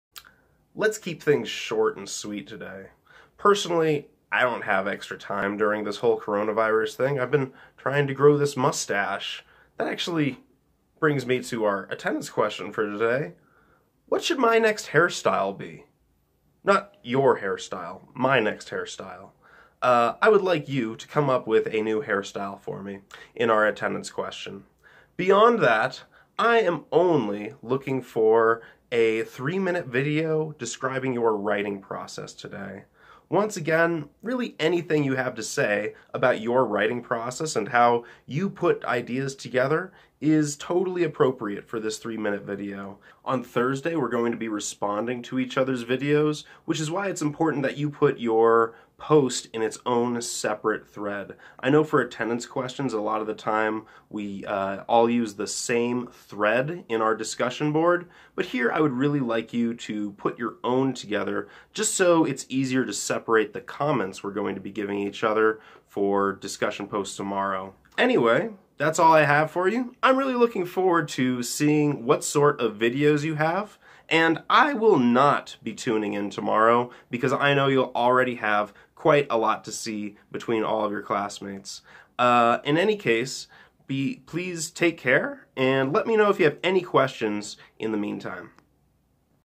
Levi Teaches Rhetoric, Day 14 -- Short and Sweet
April 22, 2020 Video Lecture for Levi's English Classes at Northern Arizona University